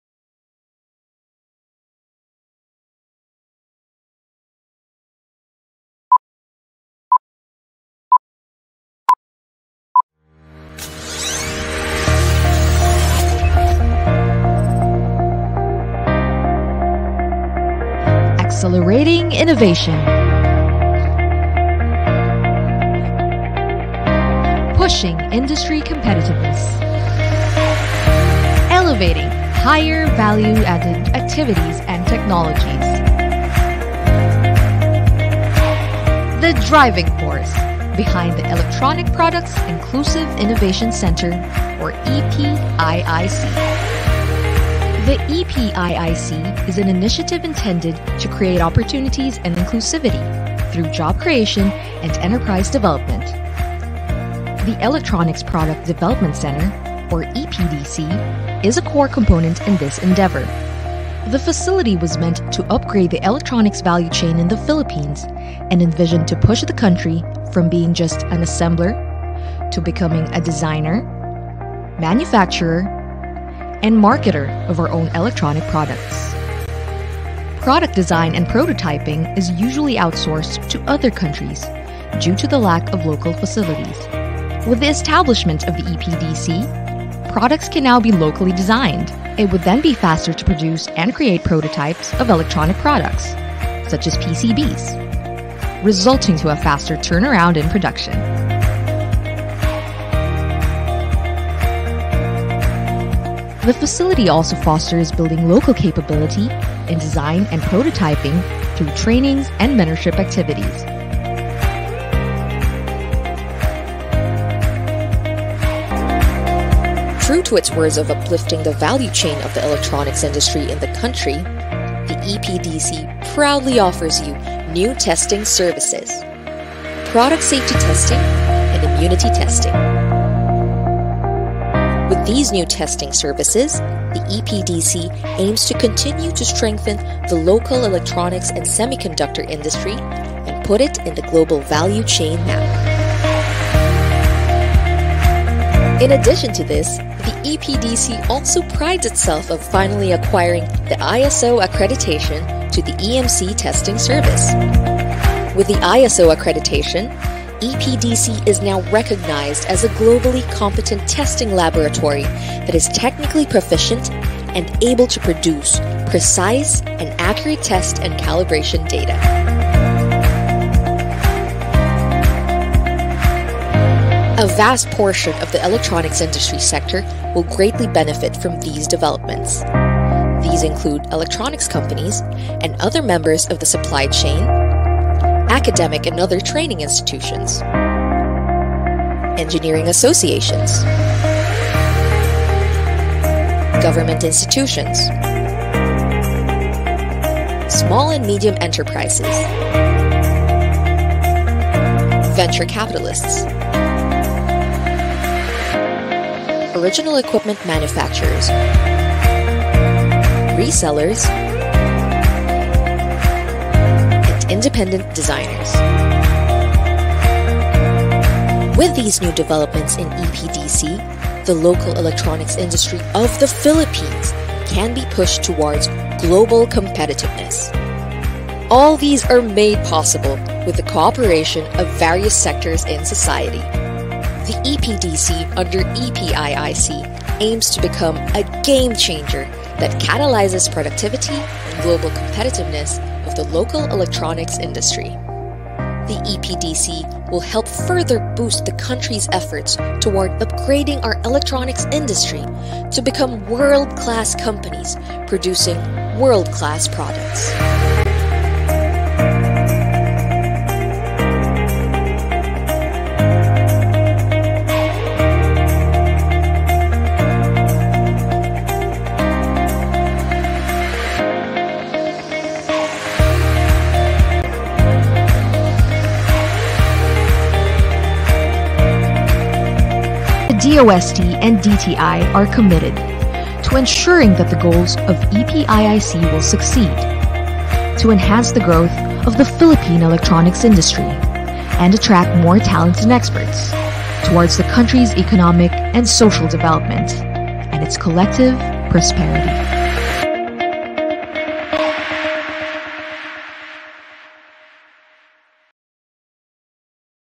PROJECT 2: Electronics Product Inclusive Innovation Center (EPIIC)
Project 2, redefines the role of the DOST-ASTI in the EPDC Program as one of research and development (R&D) aimed at spurring the growth and advancement of the EPDC as an electronics design and innovation hub. This allows the DOST-ASTI to refocus its efforts on further fulfilling its mandate as a Research and Development Institute (RDI) with a strategic pivot towards more "industry-facing" activities. One new activity will be to complement the current prototyping and EMC test capability in the EPDC with mechanisms for supplying electronic components and modules that support more efficient product ideation, design and innovation in local SMEs (and DOST-ASTI).